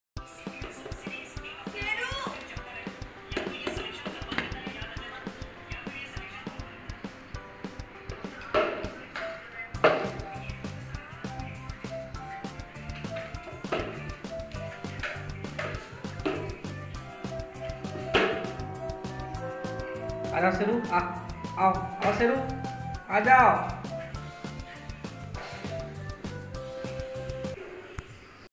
My Sheru wants to bath😂🤣....... Labrador cute puppy.
My Sheru want to bath😂🤣. Labrador cute puppy.
how to training puppy